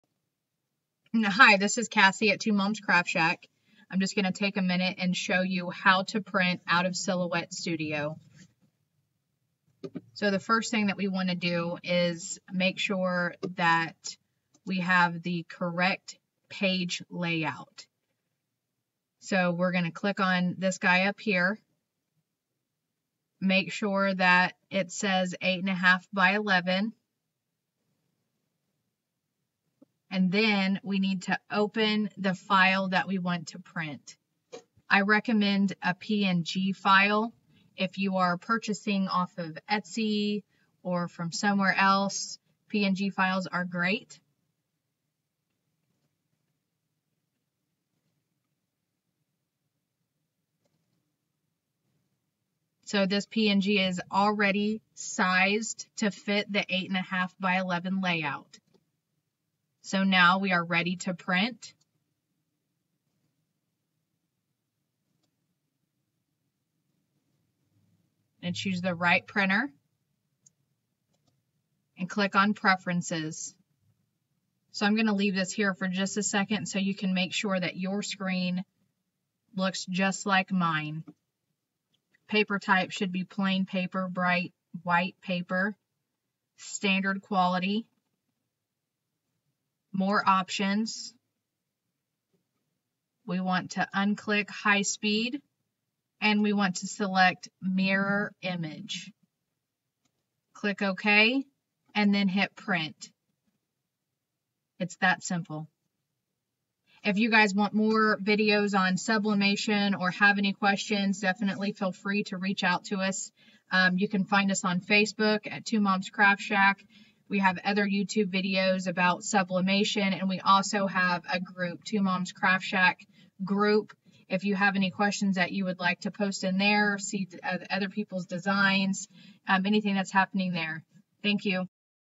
PRINT SUBLIMATION FROM SILHOUETTE STUDIO-STEP 5
How to print on your EcoTank from Silhouette Studio.

Change to high quality for the ET-2720